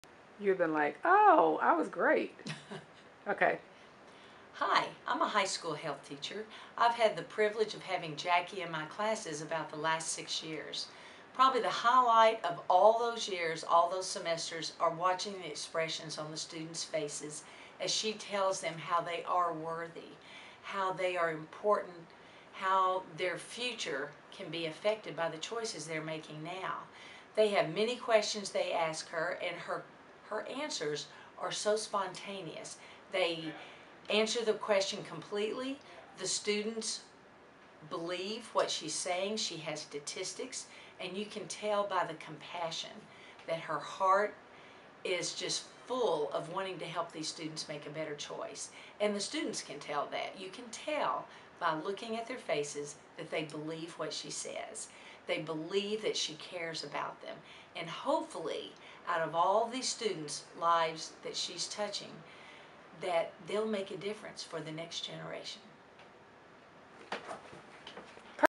Testimonial from Health Teacher
Sharon Ausenbaugh from S. Cobb High School in Austell, GA shares her experiences with Jackie Brewton speaking in her health class for the past 6 years. for information and booking.